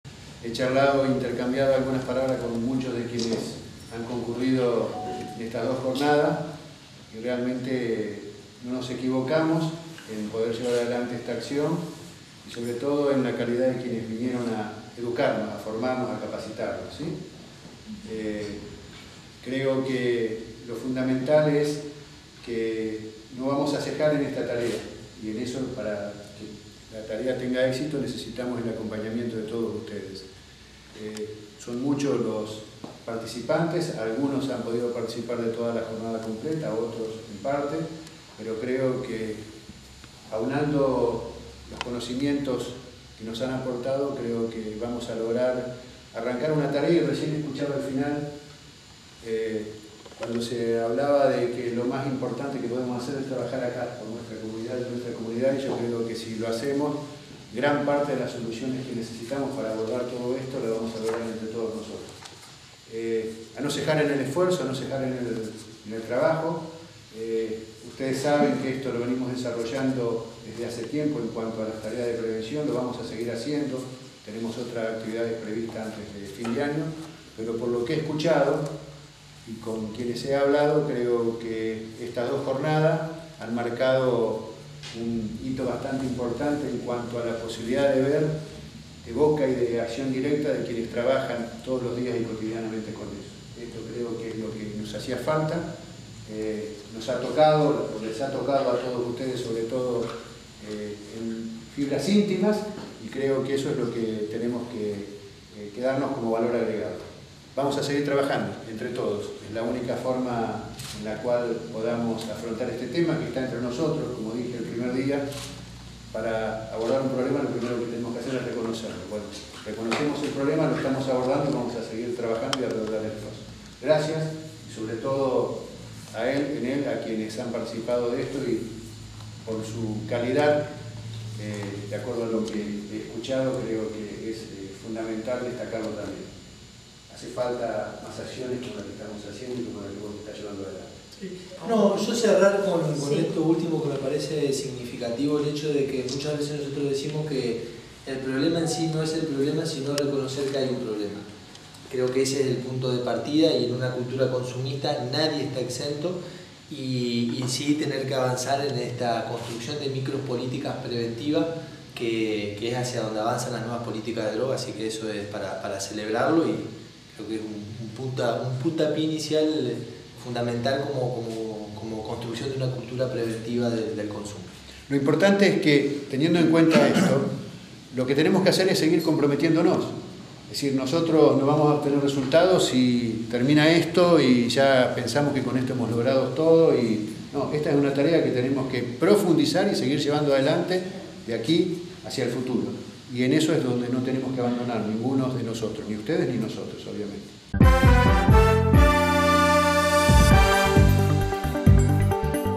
ING. JORGE PLACENZOTTI – JEFE DE GOBIERNO
SE ENTREGARON CERTIFICADOS DEL CURSO SOBRE PREVENCIÓN DE ADICCIONES